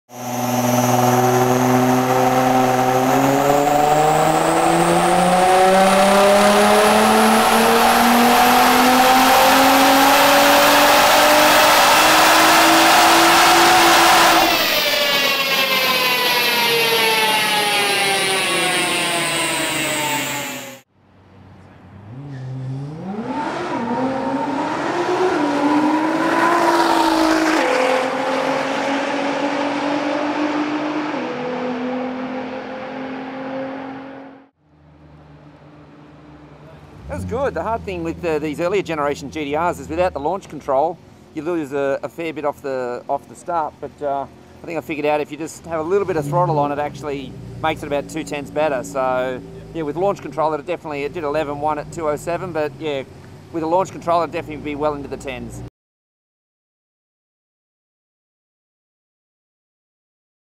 CRD Nissan GTR R35 at the 2011 Motor Hot Tuner Challenge
CRD were invited to the 2011 Motor Hot Tuner Challenge at Eastern Creek with their "hotted up" Nissan GTR R35. The Next Step sponsored GTR R35 turned many heads not only for its great looks but for its mind boggling performance described as "satanic" & "scary"! Grab your copy of the September edition of Motor magazine and read up on the awesome CRD Nissan GTR R35.

PS: Imagine what they would have thought if it wasn't detuned?

for more!